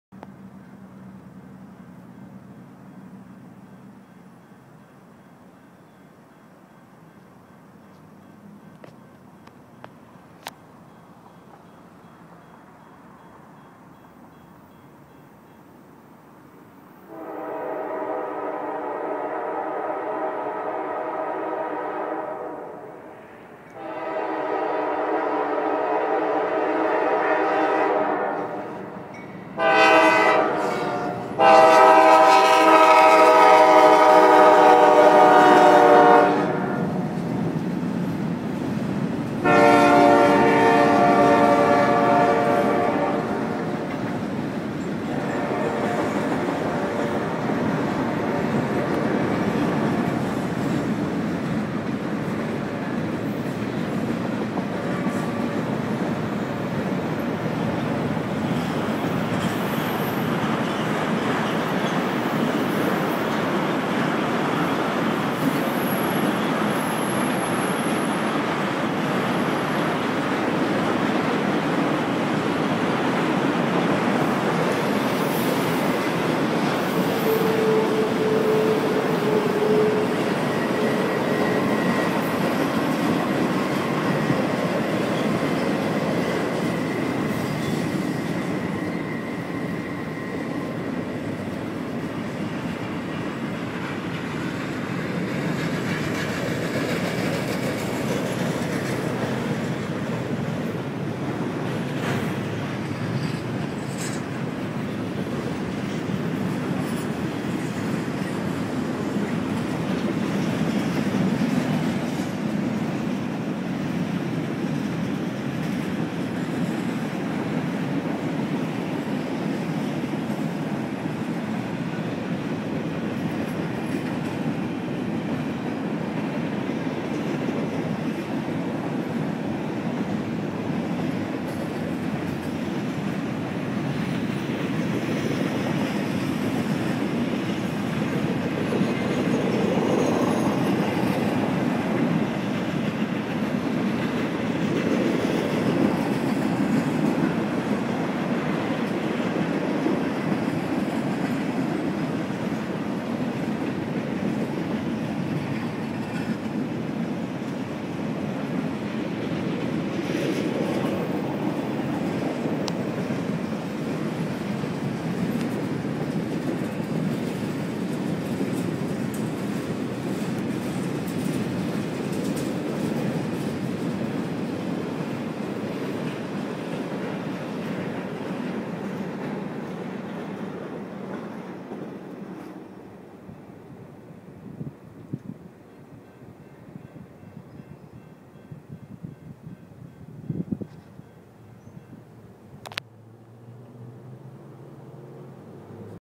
park 2-23-14 NS I24
NS I24 has just arrived at 11:15am but right after we caught NS 428. We were meeting NS 428 at the drop off sight right around the west blvd area. But anyway, NS 224 just came from Jacksonville, FL. And right now it's heading to Kansas, KS.